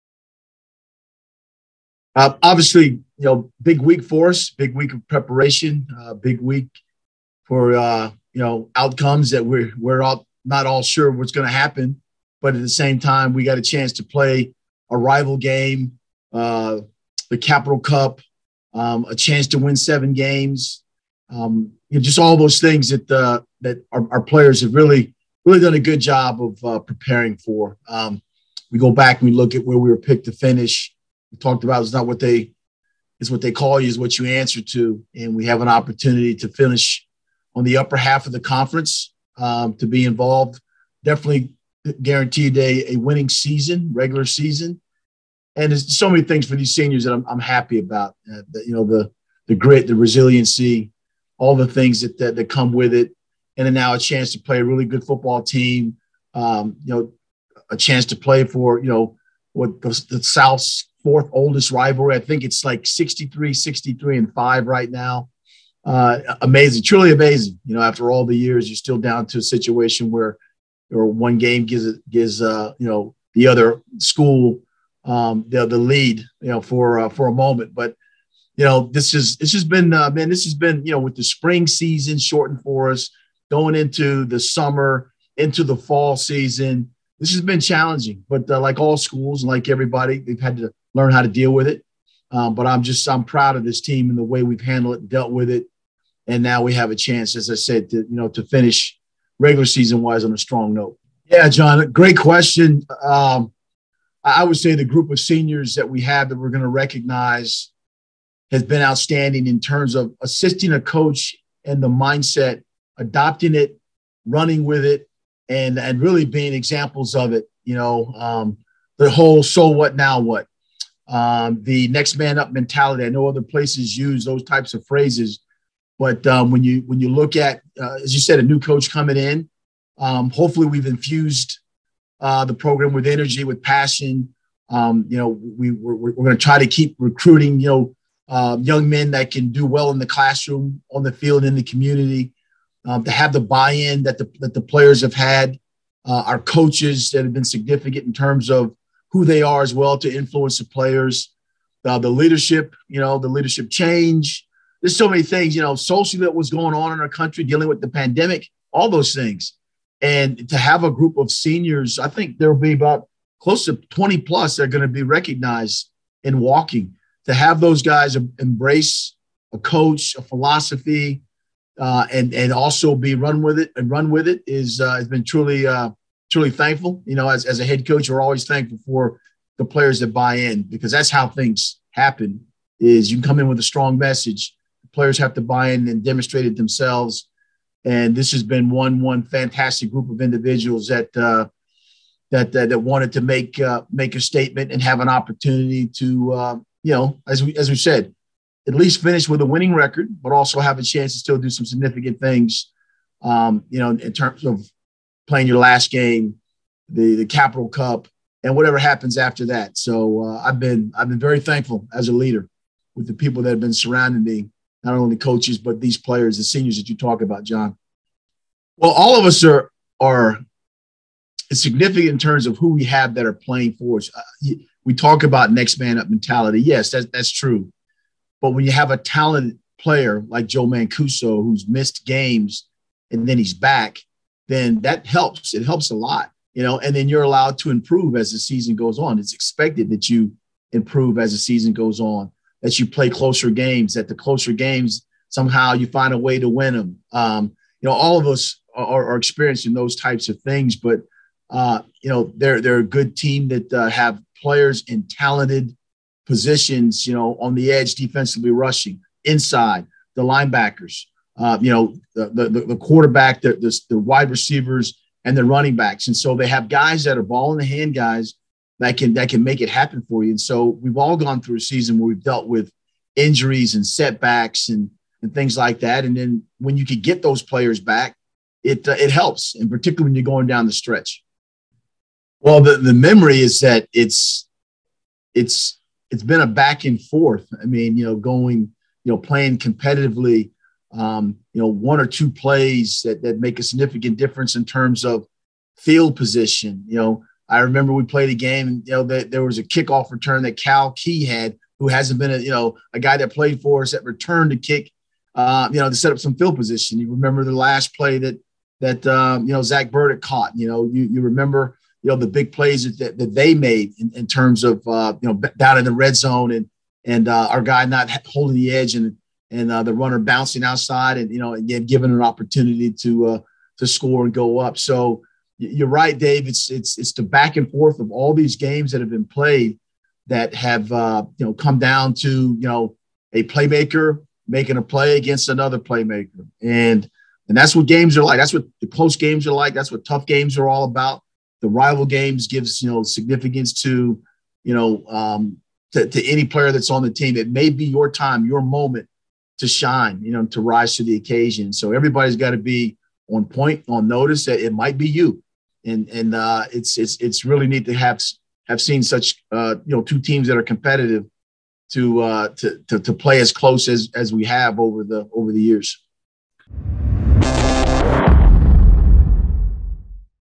2021 W&M Football - Week 11 Press Conference
William & Mary head football coach Mike London previews the Tribe's game against Richmond.